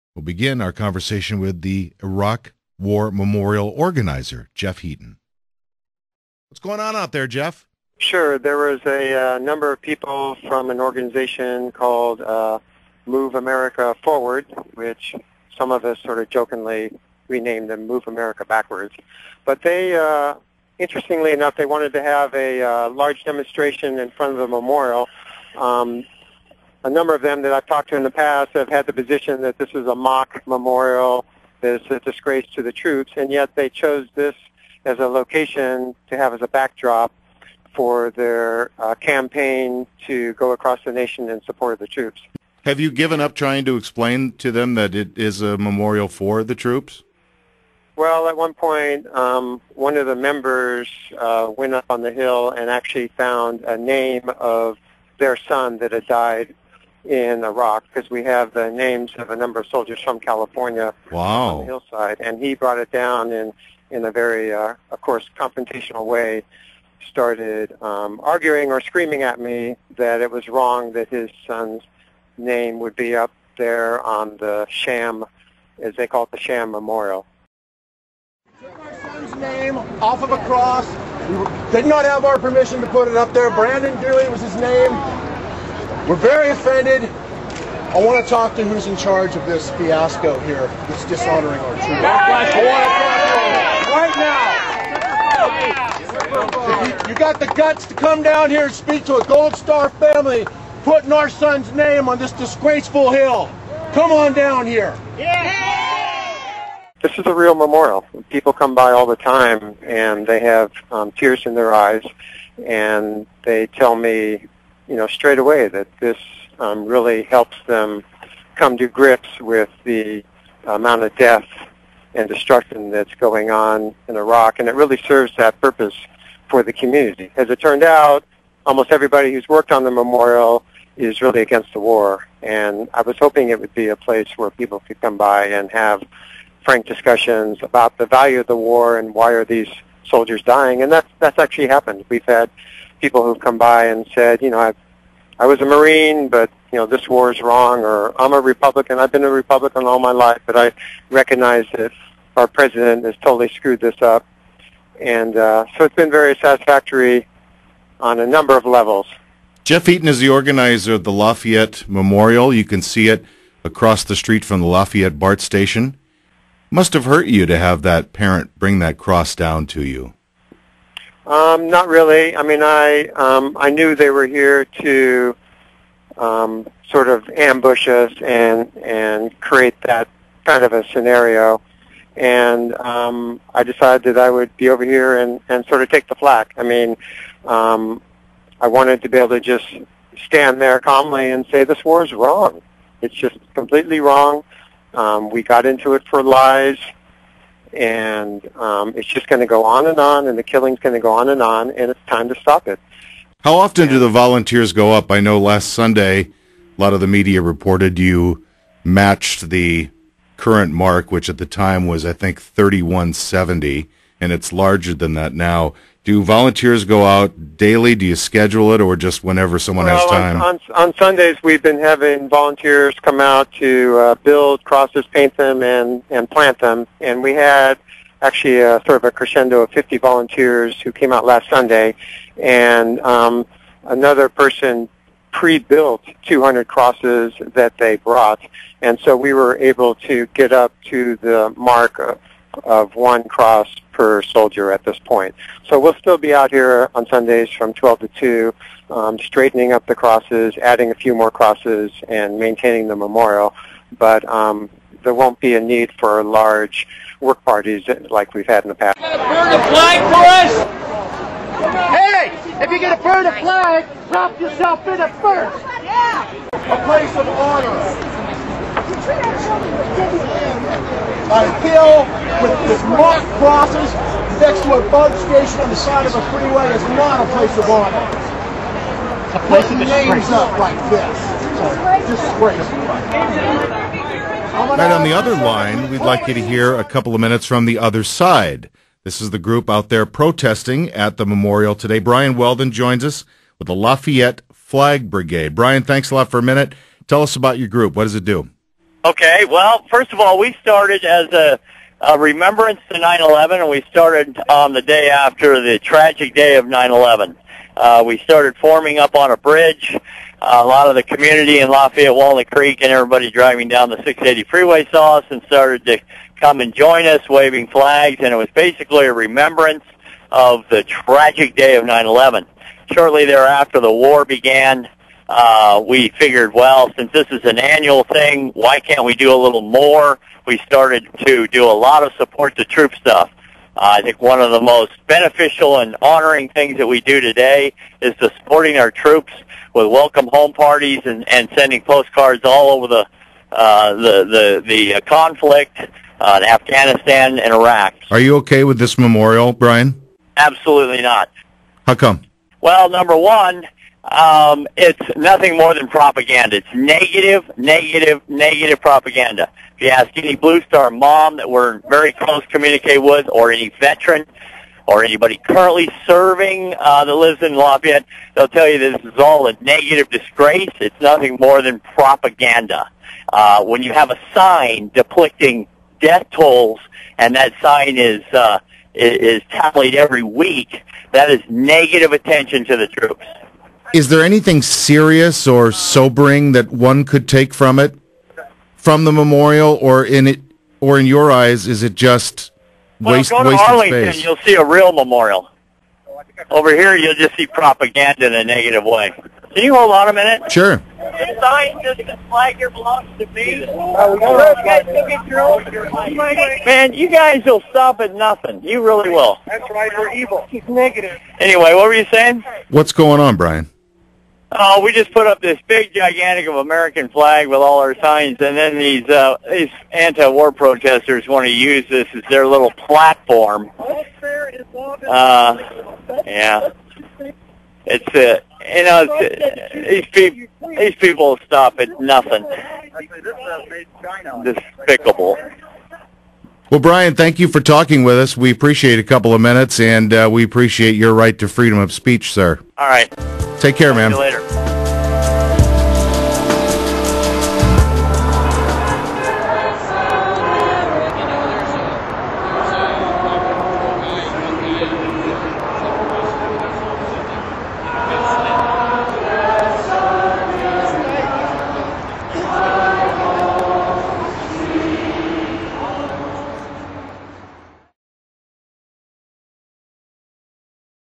crosses memorial
Crosses war memorial in San Francisco.  John Scott interviews pro-war and anti-war protesters.